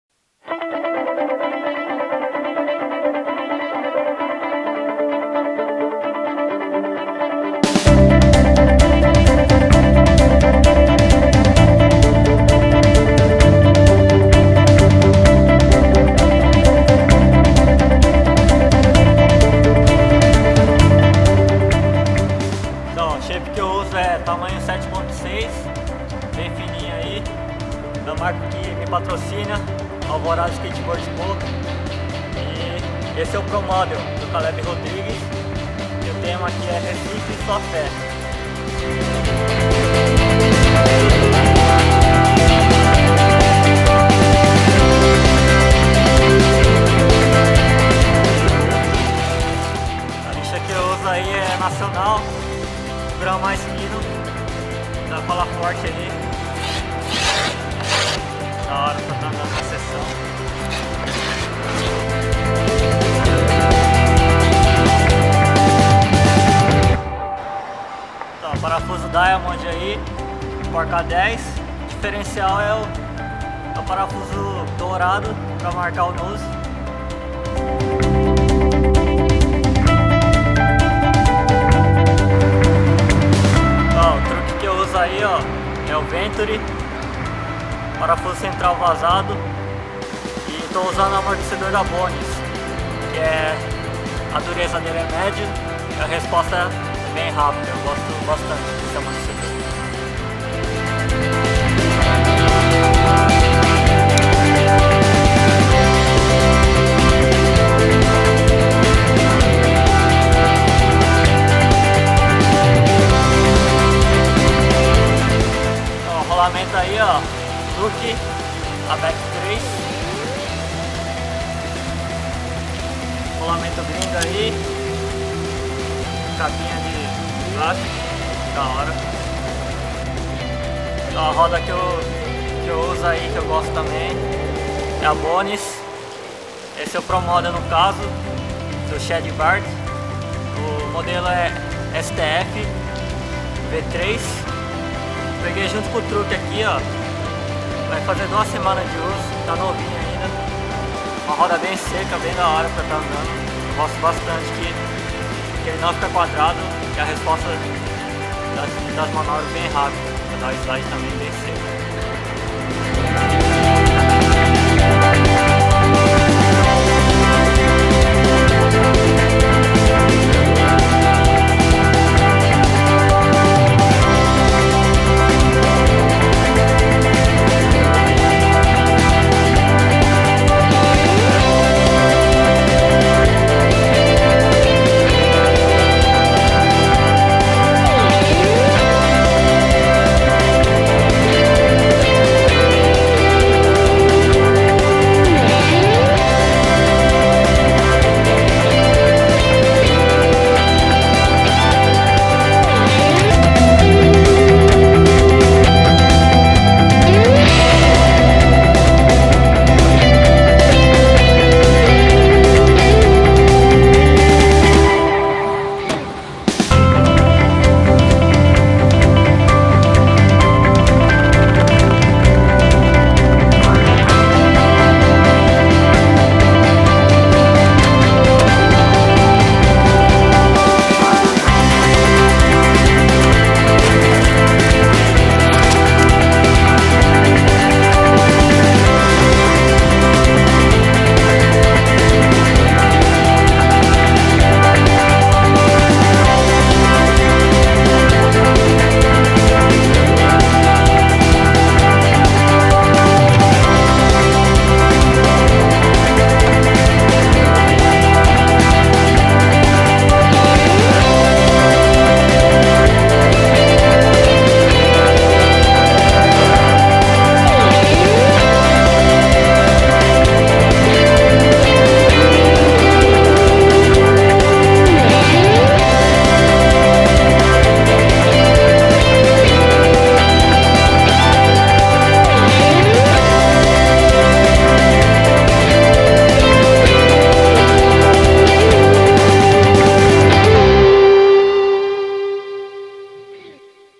MEU SKATE - Mauricio Nishihara - SKATE
► Quadro MEU SKATE ◄
Onde mostra as peças que se usa no SKATE
Nesse Vídeo Mauricio Nishihara mostra as peças e medidas que ele usa!

ALVORADA SKATEBOARD CO.